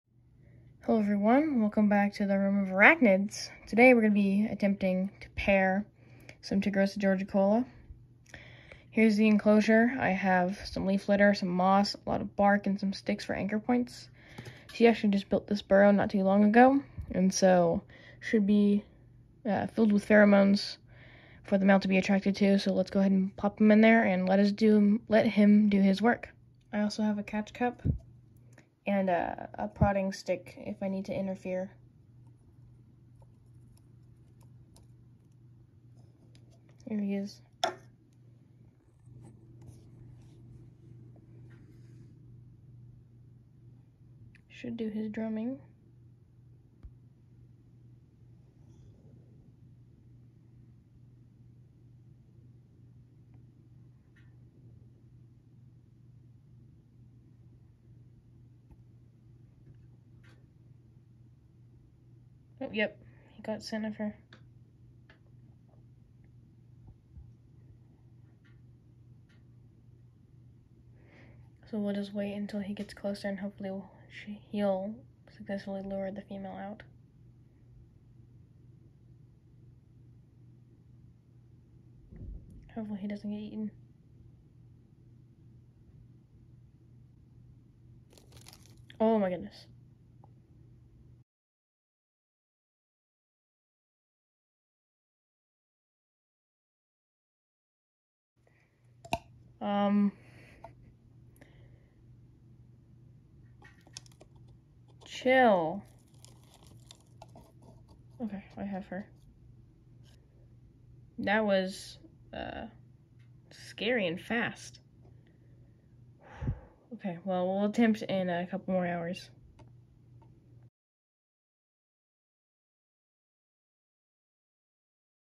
Swamp wolf spider pairing attempt (Fail)
In this video I try and pair Tigrosa georgicola (Swamp wolf spider) but the female attacks the male in a feeding response. I will probably attempt this in a couple of days.